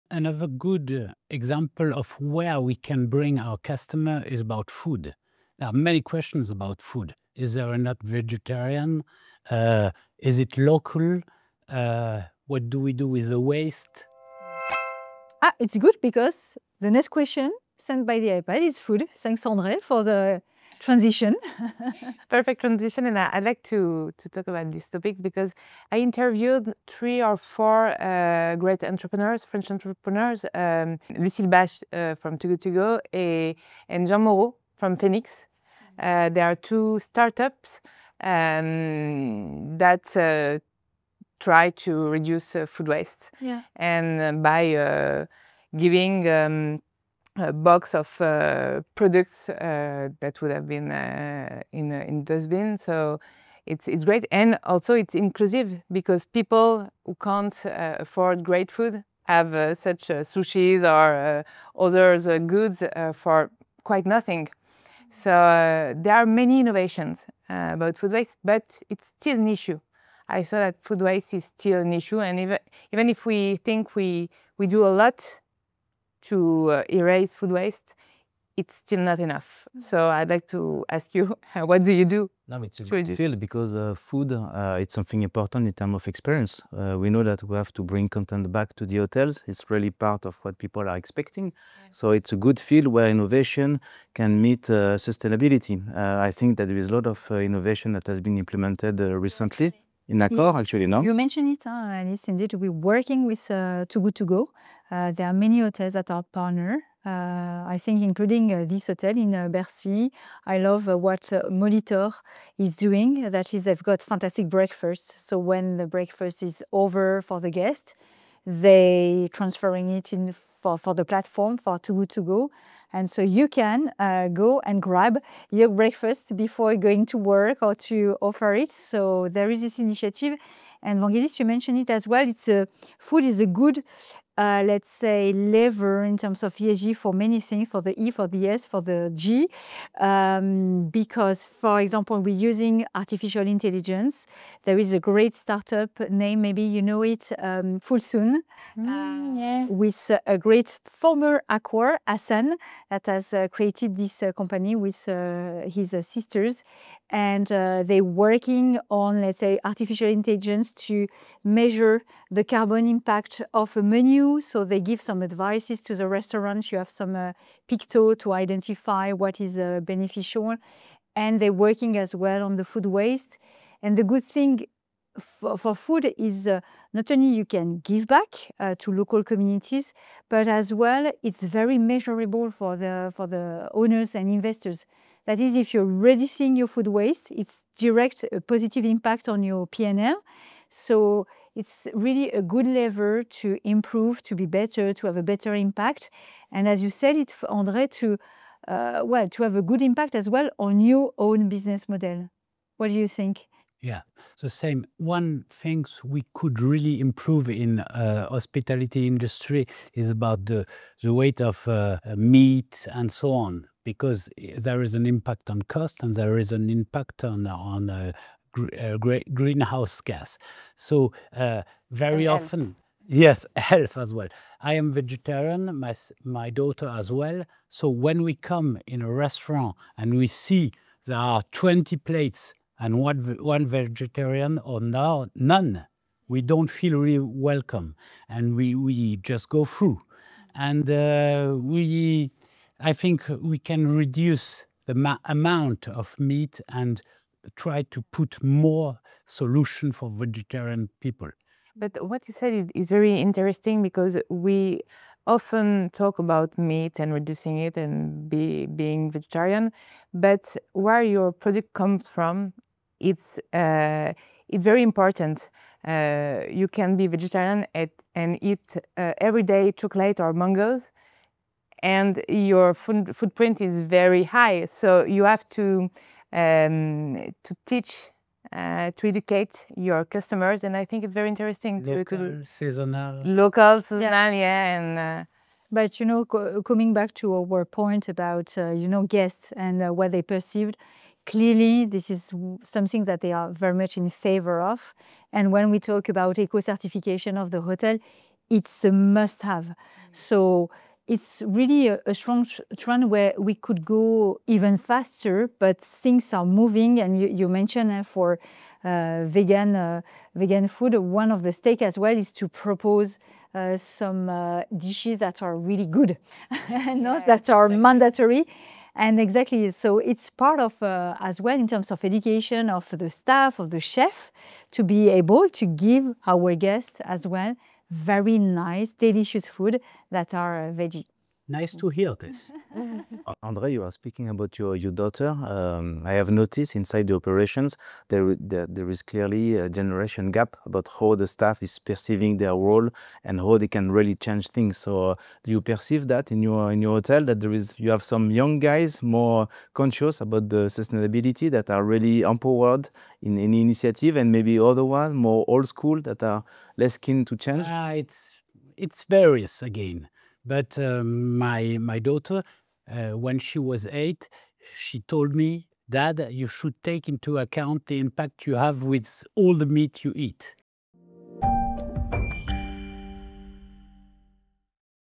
Reducing Food Waste in Hospitality: Innovations and Challenges
In this insightful discussion, thought leaders from the hospitality industry explore innovative approaches to tackling food waste. From startup initiatives like Too Good To Go to AI-powered solutions for measuring the carbon impact of menus, learn how the food sector is evolving for sustainability. Discover how restaurants and hotels are finding ways to reduce waste while improving profitability, with insights from Agnès Roquefort, Alice  Vachet, André Heintz, and Vanguelis Panayotis.